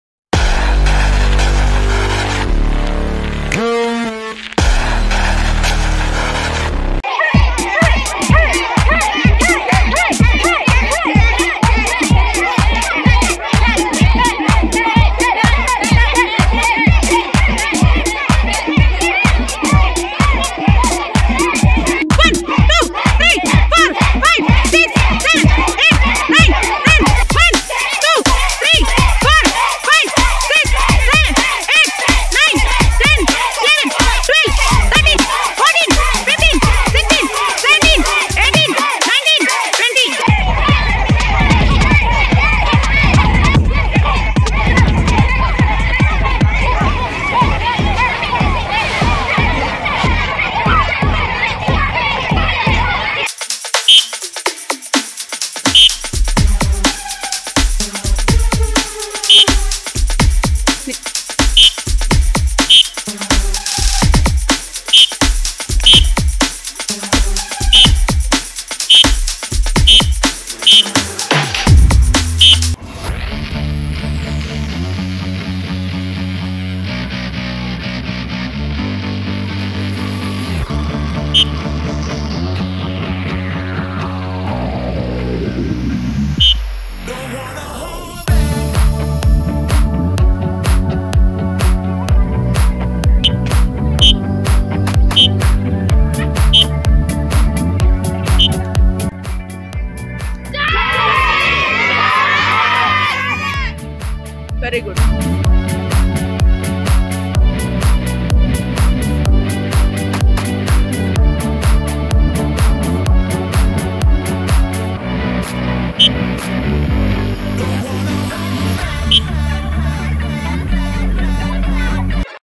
self defence skills//mission save nirbhaya//mission shakti//judo self defence//women empowerment//
hello friends... ❤❤❤❤❤

Mission save Nirbhaya. निर्भया... यह नाम तो शायद सबके जहन में अभी भी होगा. बहुत से मुश्किलों से लड़ कर भी निर्भया अपने आप को बचा नहीं सकी. पर हमें निर्भया जैसी बहुत सी ऐसी लड़कियों को इन मुश्किलों से लड़ना सिखाना है... क्योंकि इंसान की कोशिश कभी जाया नहीं जाती कब क्या होने वाला यह कोई नहीं बता सकता पर हमें मुश्किलों से लड़ने के लिए तैयार रहना है. दोस्तों अगर आप सभी को लगता है कि मेरा यह काम अच्छा है और हमें बच्चों को सुरक्षा प्रदान करनी चाहिए प्लीज प्लीज मेरे चैनल को लाइक करें सब्सक्राइब करें और शेयर करें आप सभी के सहयोग से वीडियो में दी जाने वाली जानकारी आगे जाएंगी. 🙏🙏🙏🙏